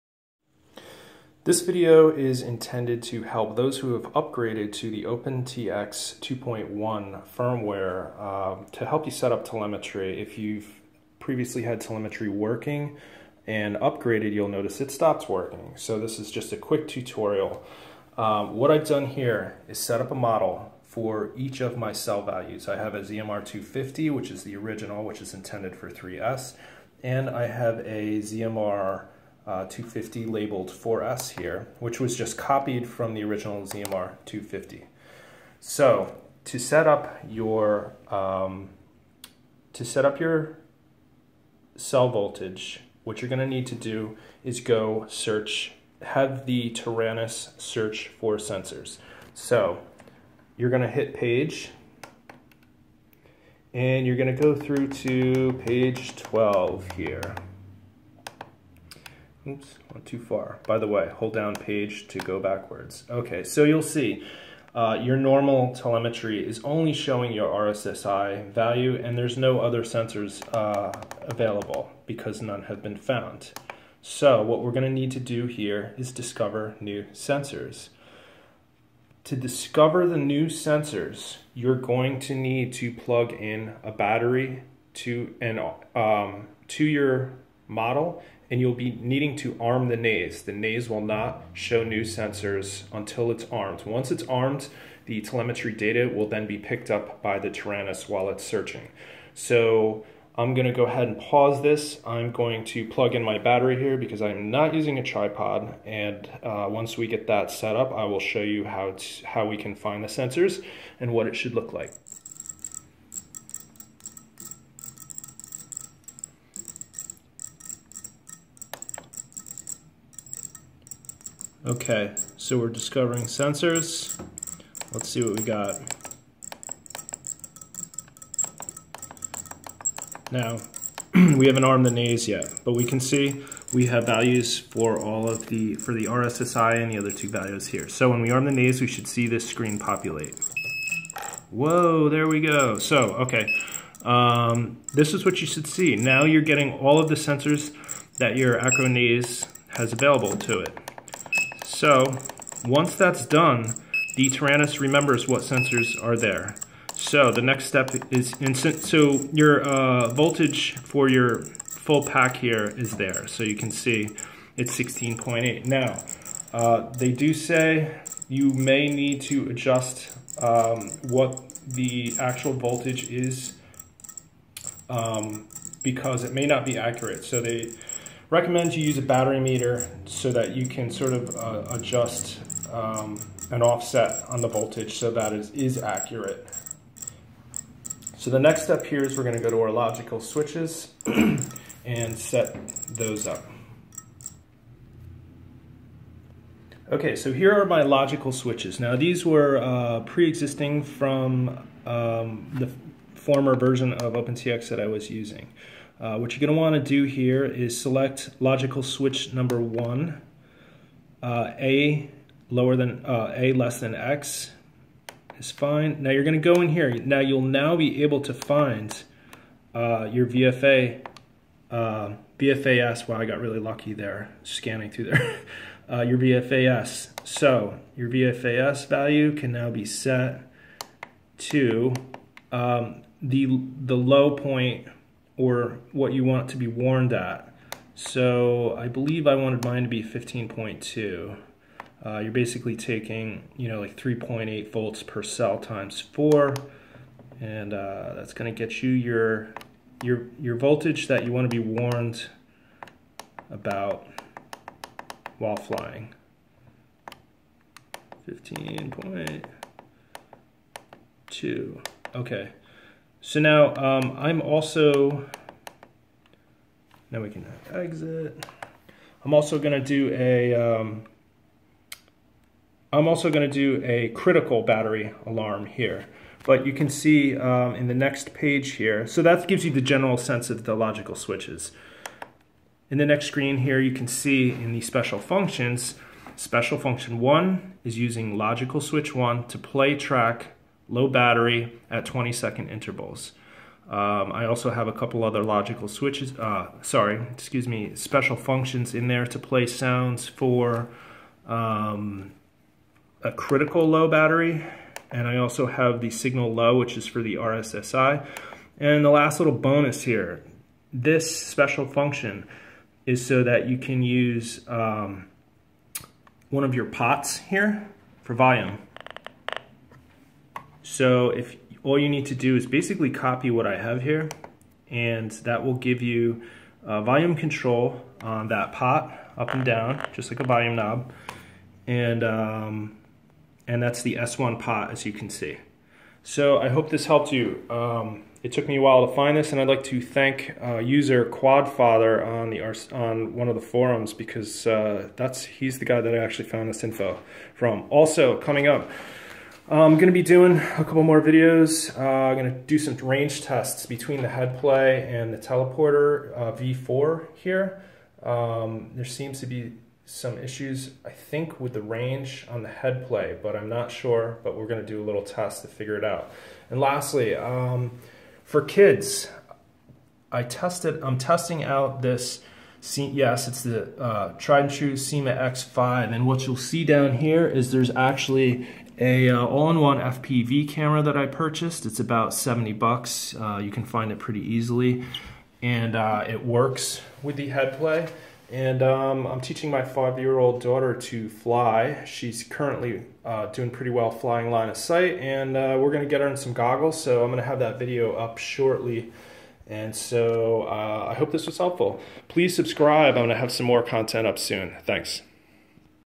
How to setup Telemetry on the Taranis OpenTX 2.1 for low voltage alarms
This is a quick tutorial on how to configure your low voltage alarms in OpenTx 2.1. This video shows how the Taranis pulls information from the Naze32 Acro board once it is armed.